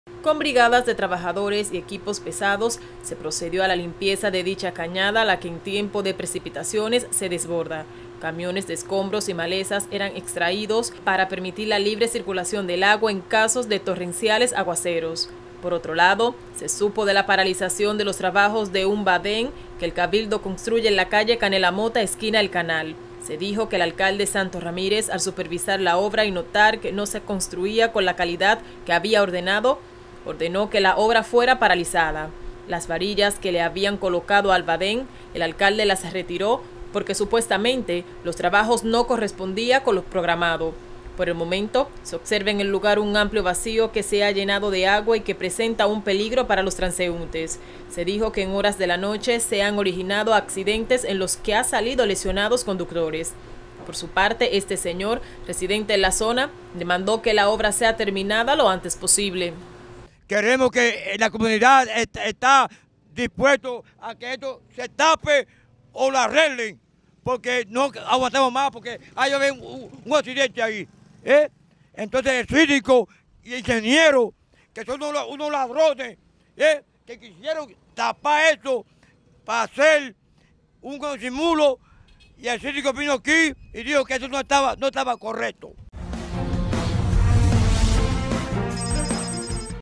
AYUNTAMIENTO LIMPIA CAÑADA DEL 30 DE MAYO
El ayuntamiento municipal de Baní realizó un operativo de limpieza en la cañada del barrio 30 de mayo.